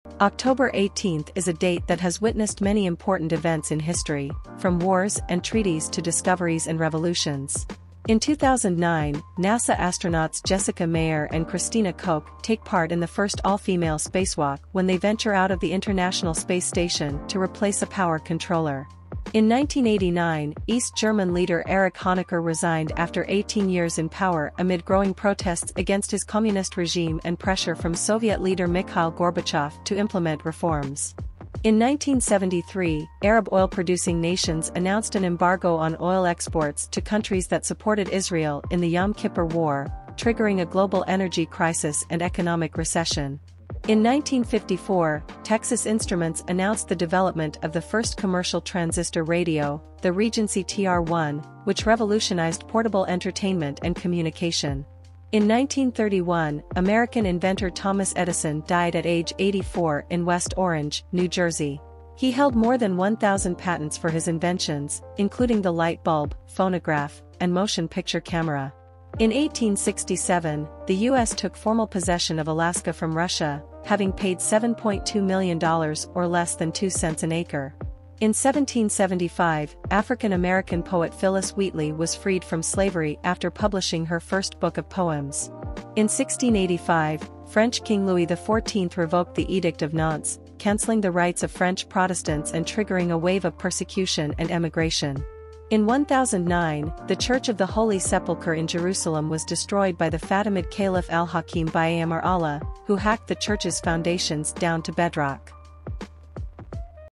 On This Day In History - 18th October
October 18th is a date that has witnessed many important events in history, from wars and treaties to discoveries and revolutions.

- In 1009, the Church of the Holy Sepulchre in Jerusalem was destroyed by the Fatimid caliph Al-Hakim bi-Amr Allah, who hacked the Church's foundations down to bedrock.
- In 1685, French King Louis XIV revoked the Edict of Nantes, cancelling the rights of French Protestants and triggering a wave of persecution and emigration.
- In 1775, African American poet Phillis Wheatley was freed from slavery after publishing her first book of poems.
- In 1867, the US took formal possession of Alaska from Russia, having paid $7.2 million or less than two cents an acre.
- In 1931, American inventor Thomas Edison died at age 84 in West Orange, New Jersey. He held more than 1,000 patents for his inventions, including the light bulb, phonograph, and motion picture camera.
- In 1954, Texas Instruments announced the development of the first commercial transistor radio, the Regency TR-1, which revolutionized portable entertainment and communication.
- In 1973, Arab oil-producing nations announced an embargo on oil exports to countries that supported Israel in the Yom Kippur War, triggering a global energy crisis and economic recession.
- In 1989, East German leader Erich Honecker resigned after 18 years in power amid growing protests against his communist regime and pressure from Soviet leader Mikhail Gorbachev to implement reforms.
- In 2009, NASA Astronauts Jessica Meir and Christina Koch take part in the first all-female spacewalk when they venture out of the International Space Station to replace a power controller.